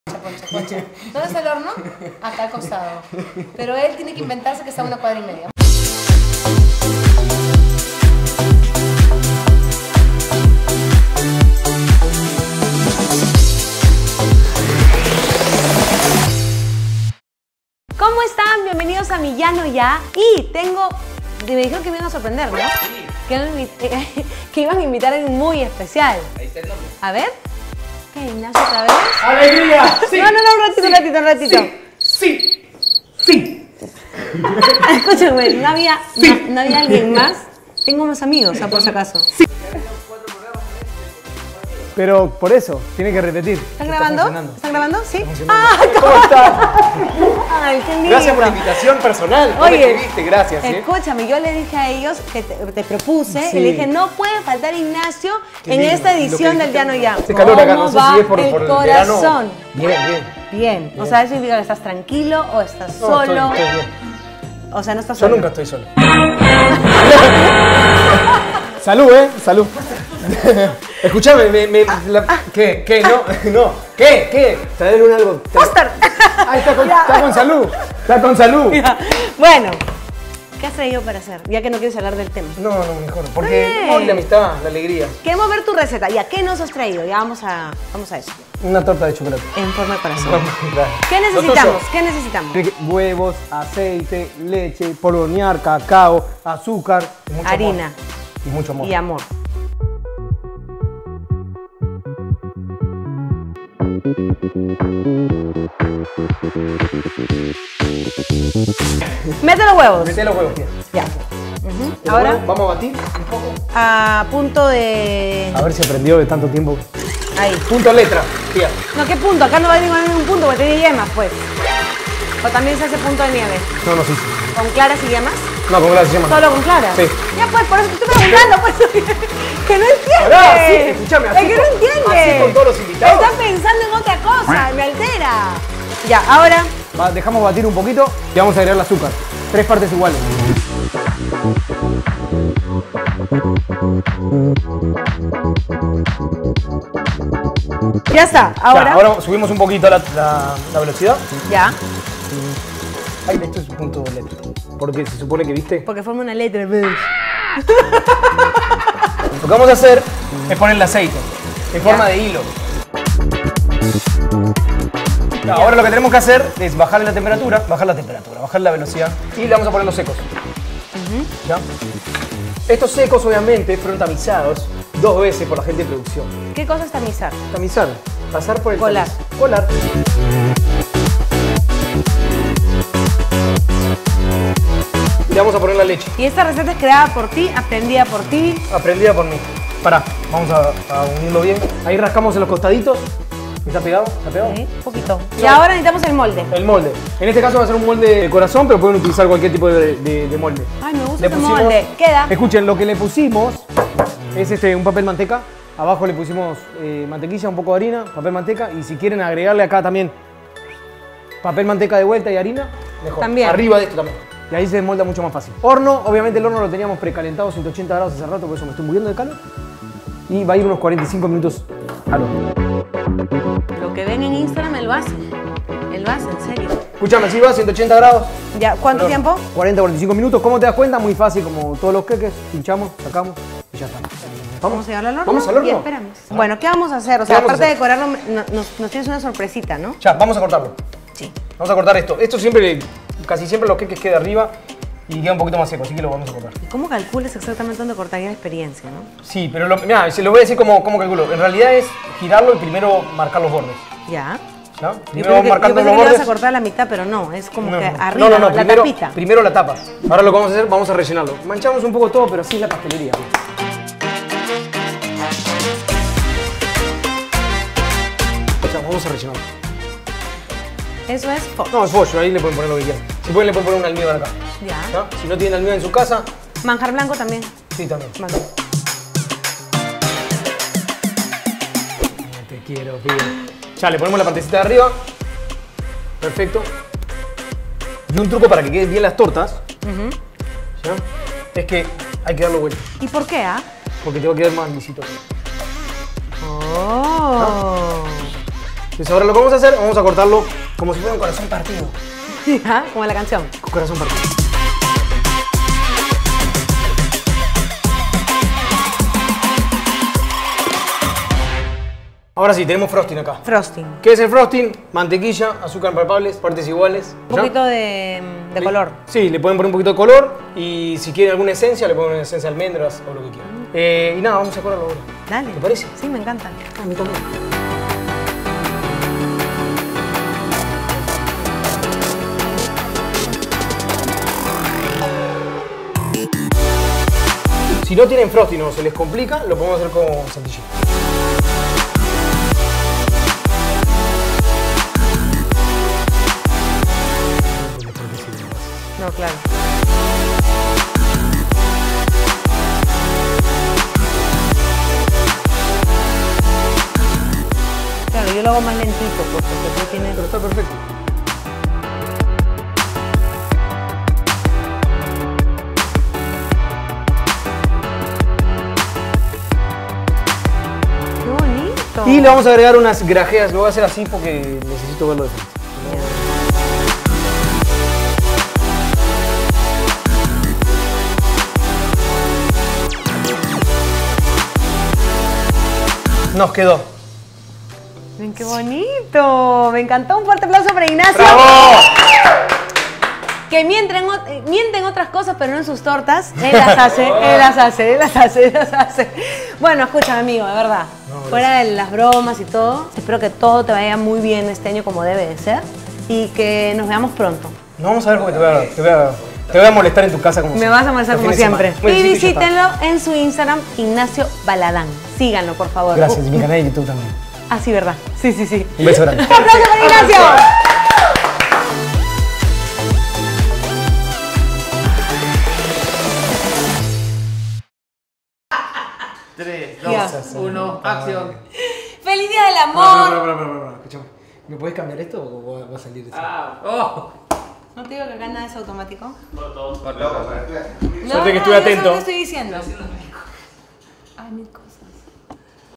YaNoYa: Torta de chocolate con Ignacio Baladan - Pia Copello
Ustedes lo pidieron en los comentarios de PreguntasSinFiltro y aquí lo tienen: Ignacio Baladan me acompaña y nos enseña a preparar una torta de chocolate